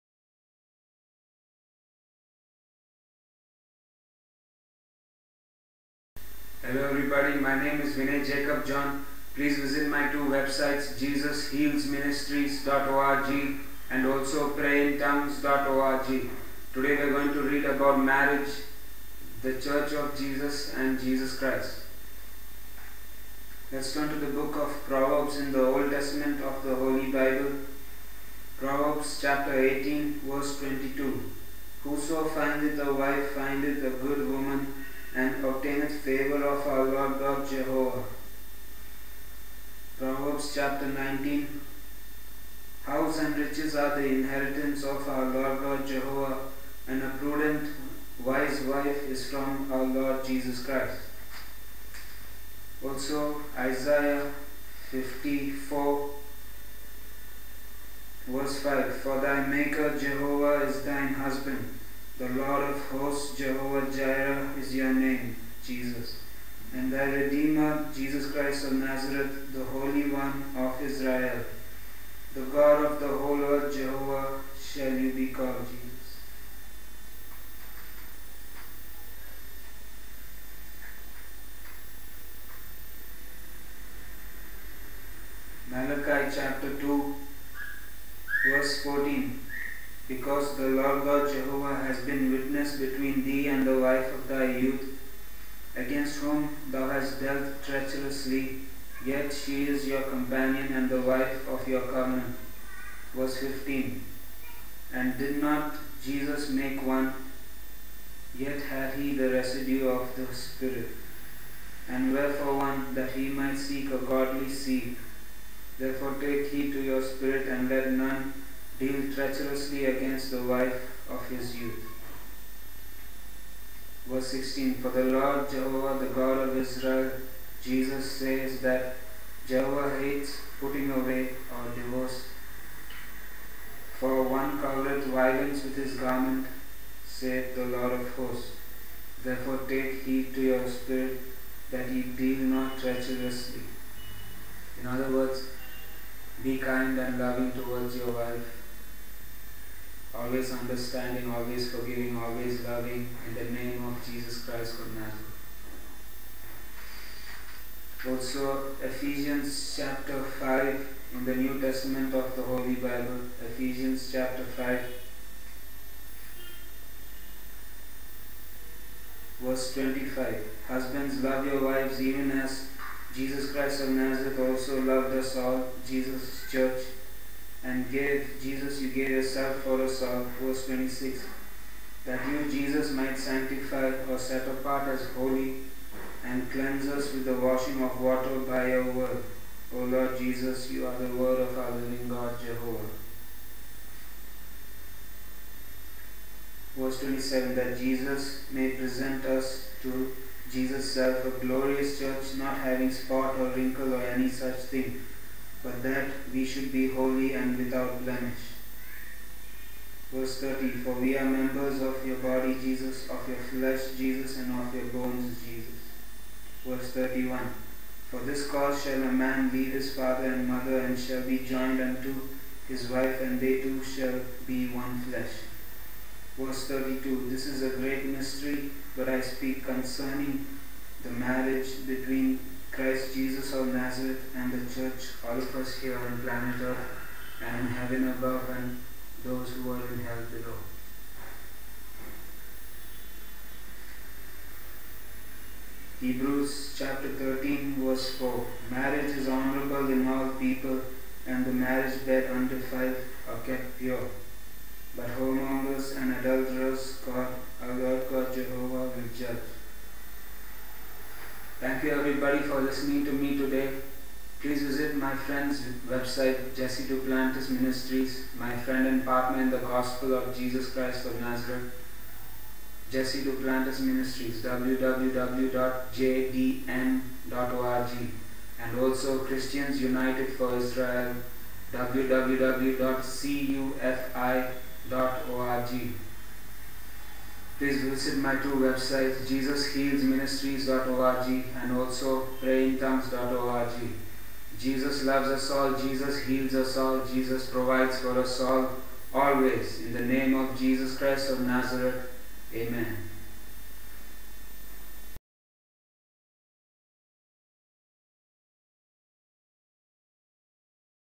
Marriage of Jesus and Us All the Church !!!! Word for the Day !!!! - 10th Saturday December 2011!!!!
HELLO EVERYBODY HERE at Facebook and Youtube !

THIS VIDEO IS ME PROPHET EVANGELIST PASTOR APOSTLE VINAY JACOB

JOHN  READING FROM THE HOLY BIBLE FROM   -     Proverbs 18: 22

Proverbs 19: 14                Isaiah 54: 5                      Malachi 2: 14 to 16

Ephesians 5: 25 to 32 and Hebrews 13: 4 !